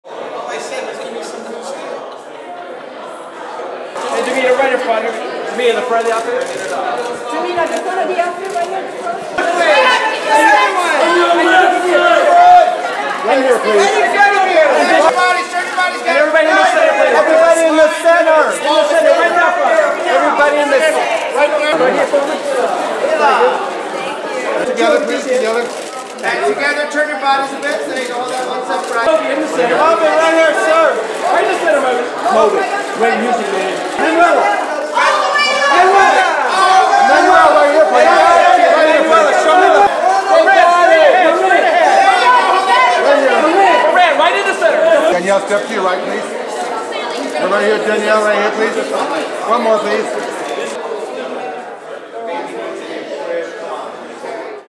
EVENT CAPSULE CLEAN - 'Third Person' Los Angeles Premiere
EVENT CAPSULE CLEAN - 'Third Person' Los Angeles Premiere at Linwood Dunn Theater at the Pickford Center for Motion Study on June 09, 2014 in Hollywood, California.

Featuring:
    Kristanna Loken
    Mila Kunis
    Tehmina Sunny
    Maria Bello
    Moran Atias
    Moby
    Emanuela Postacchini
    Danielle Vasinova